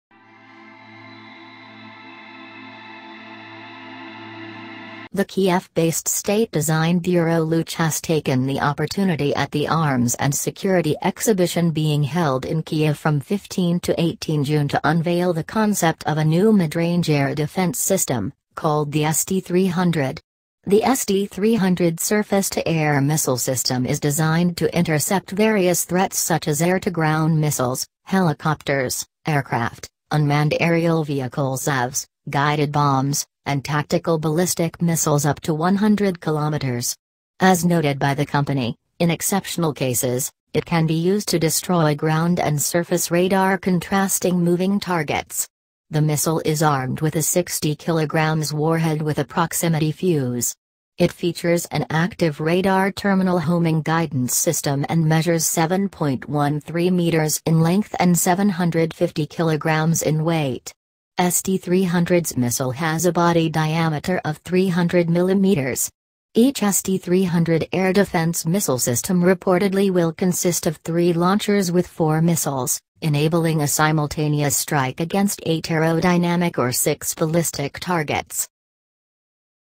Is the Ukrainian Air Defense System SD-300 copy of S-300?
The Kyiv-based state design bureau LUCH has taken the opportunity at the Arms and Security exhibition being held in Kyiv from 15 to 18 June to unveil the concept of a new mid-range air defense system, called the SD-300. The SD-300 surface-to-air missile system is designed to intercept various threats such as air-to-ground missiles, helicopters, aircraft, unmanned aerial vehicles UAVs, guided bombs, and tactical ballistic missiles up to 100km. As noted by the company, in exceptional cases, it can be used to destroy ground and surface radar-contrasting moving targets. The missile is armed with a 60 kg warhead with a proximity fuse. It features an active radar terminal homing guidance system and measures 7.13m in length and 750kg in weight. SD-300’s missile has a body diameter of 300mm. Each SD-300 air-defense missile system reportedly will consist of 3 launchers with 4 missiles, enabling a simultaneous strike against 8 aerodynamic or 6 ballistic targets.